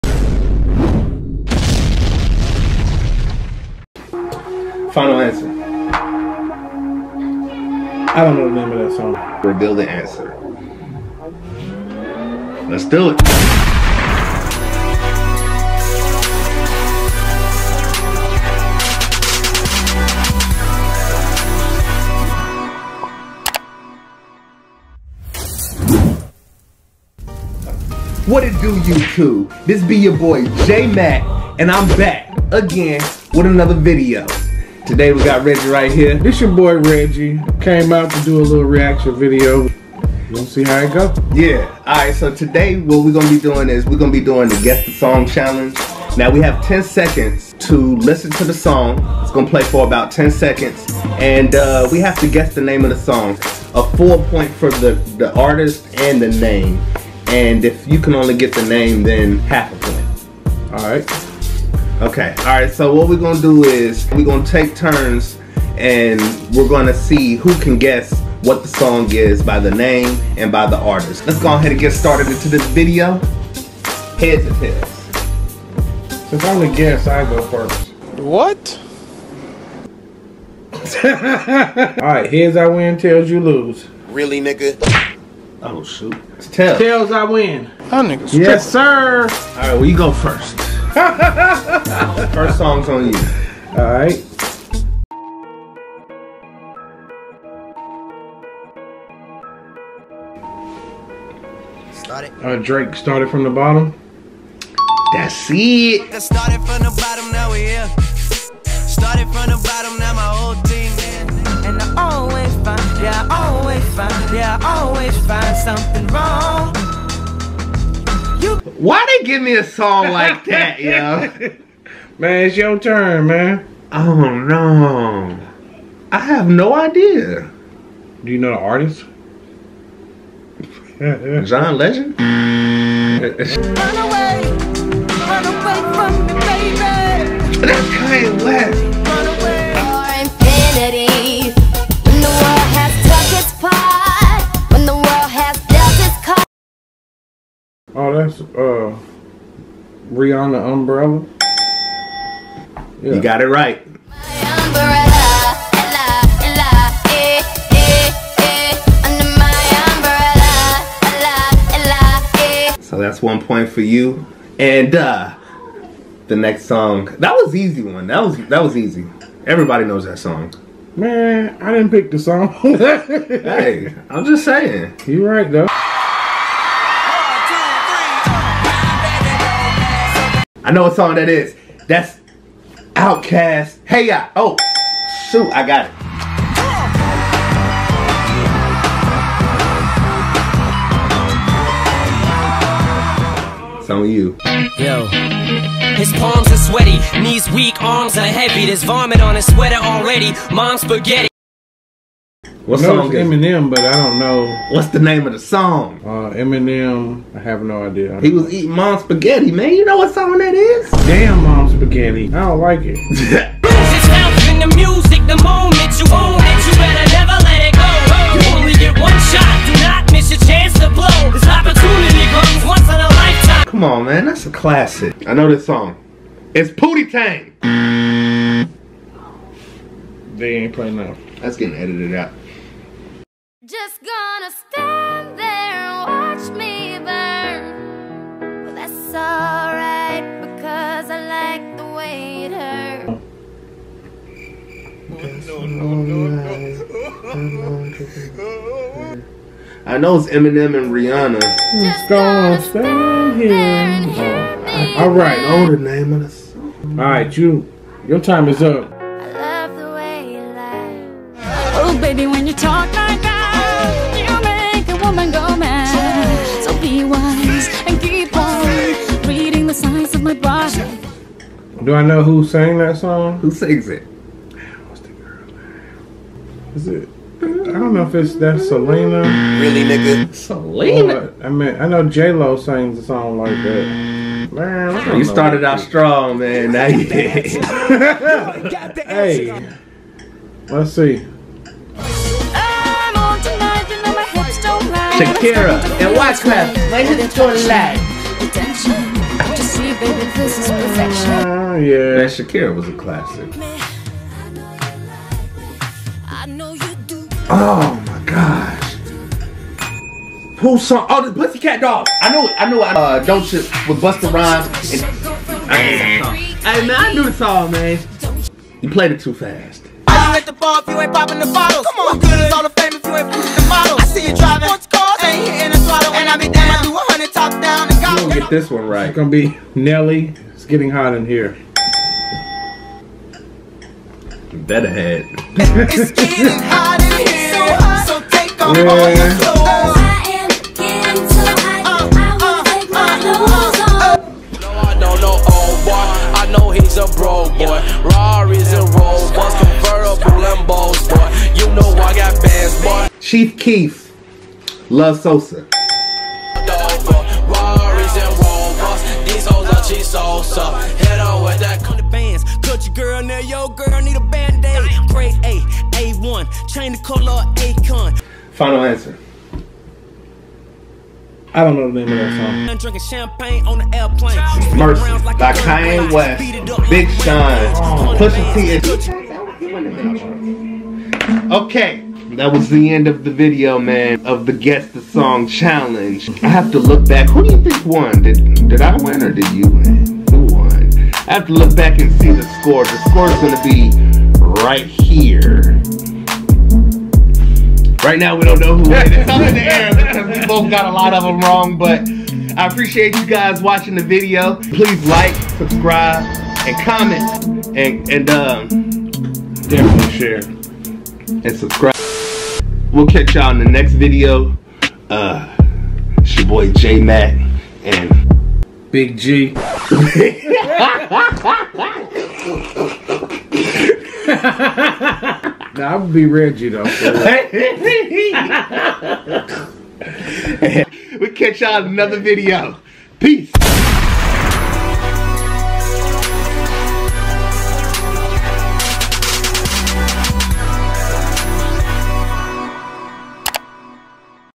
Guess The Song Challenge (Hip-Hop/R&B/Pop Edition)
Today I Decided To Do The Guess The Song Challenge With Reggie From "Double G TV".